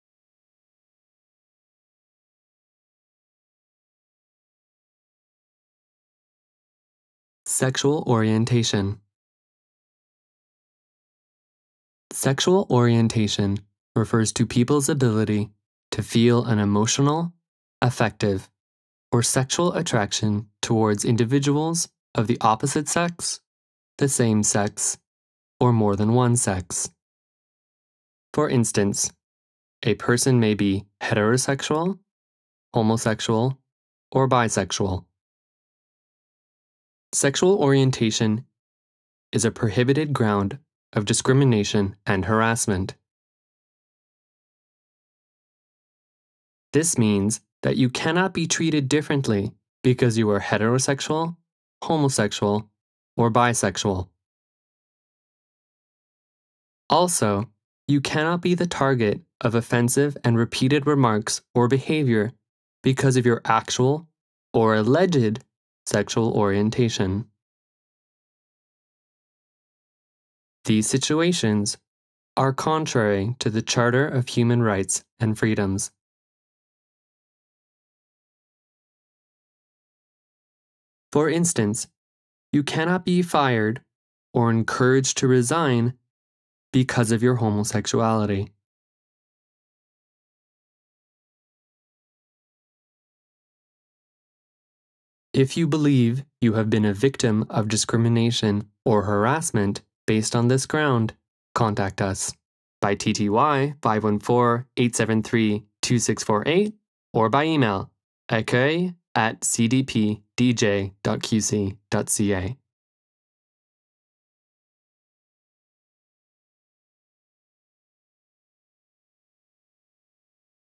Ground #8 Sexual orientation
In this video in American Sign Language (ASL), the Commission explains that a person's sexual orientation represents a forbidden ground of discrimination and harassment in Québec under the Quebec Charter of Human Rights and Freedoms.